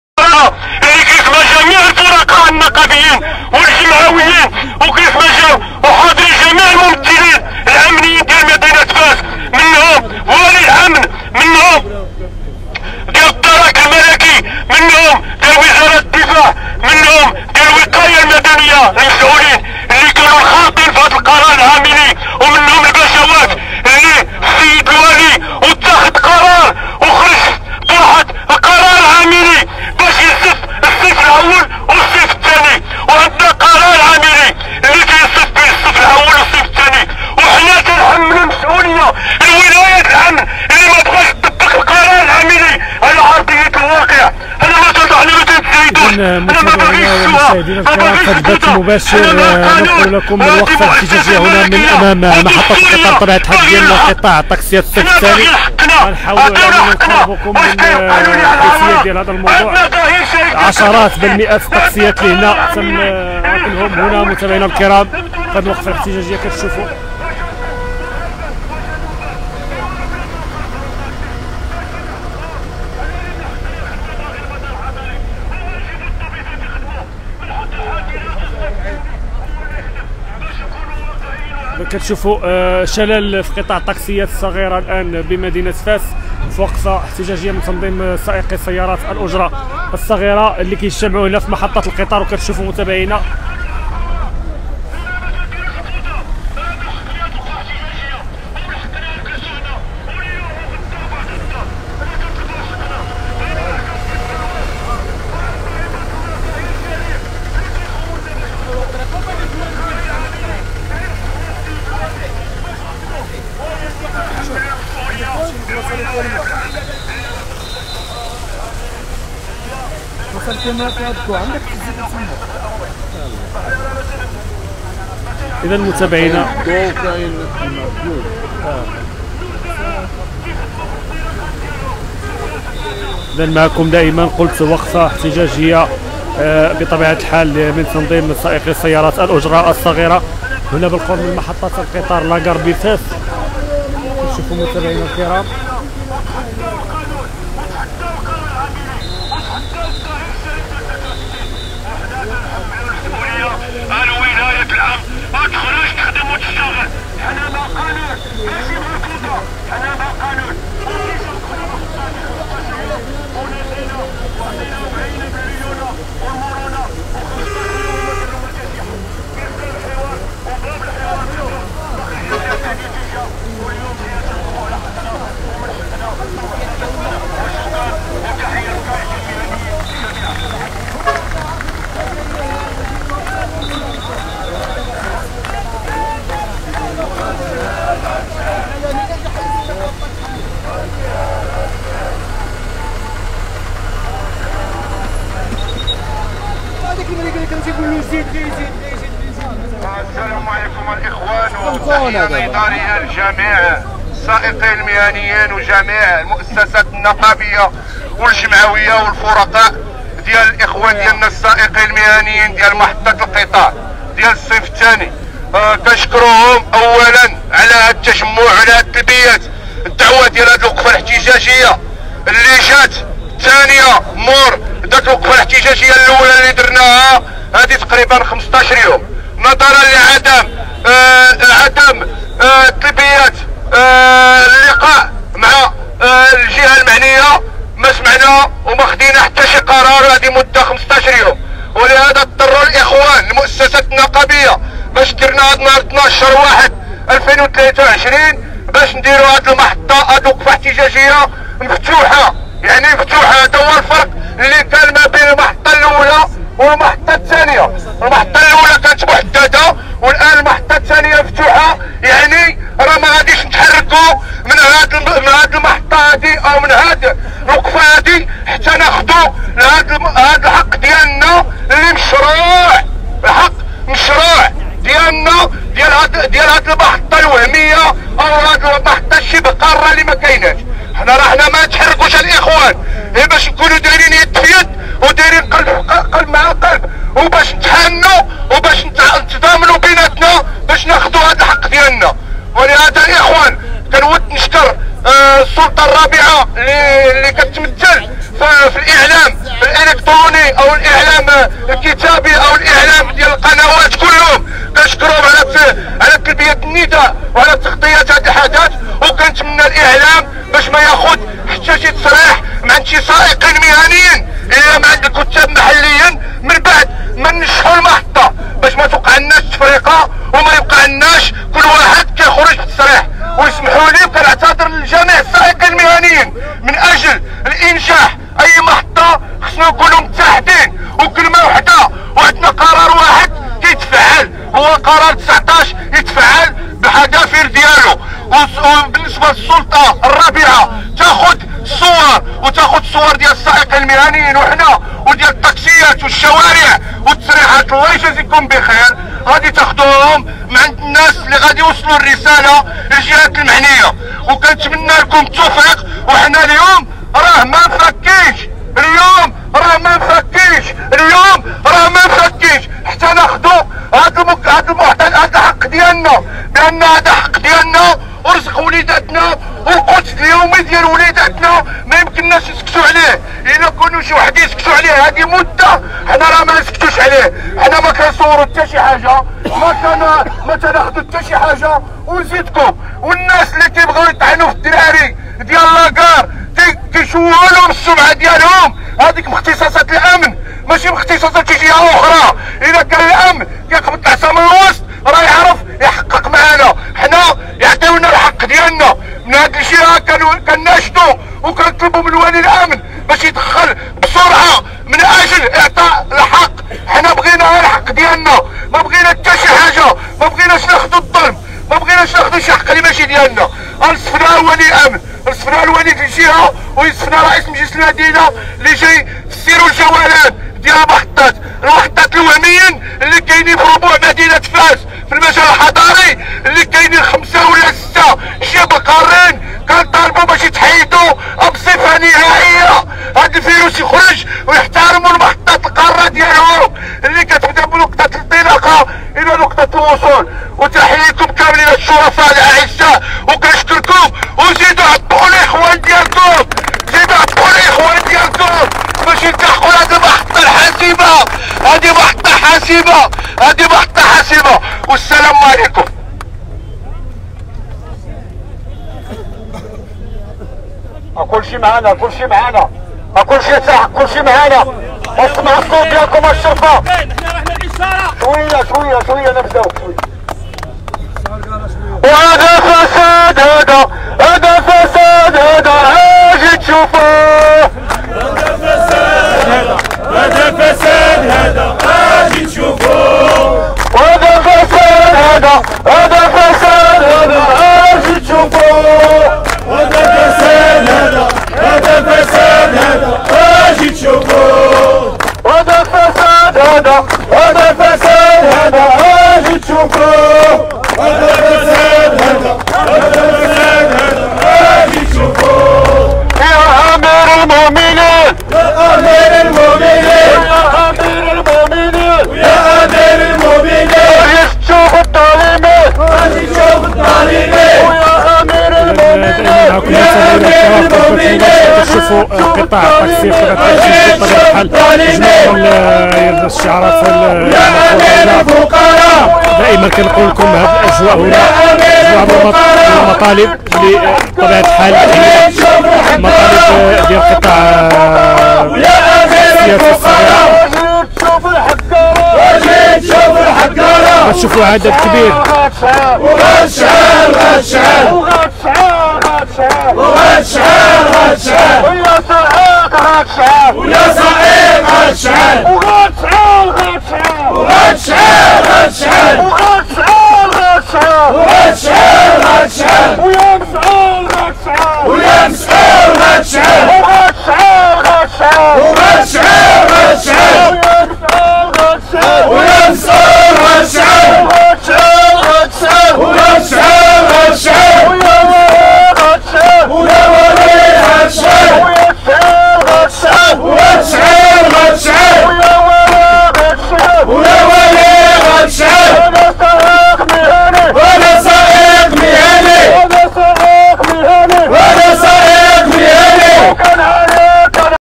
سائقو سيارات الأجرة الصغيرة منظمين وقفة احتجاجية فهاد اللحظات بالعاصمة العلمية:بغينا الحقوق ديالنا
مباشرة من فاس: نايضة.. سائقو سيارات الأجرة الصغيرة منظمين وقفة احتجاجية فهاد اللحظات بالعاصمة العلمية: "بغينا الحقوق ديالنا"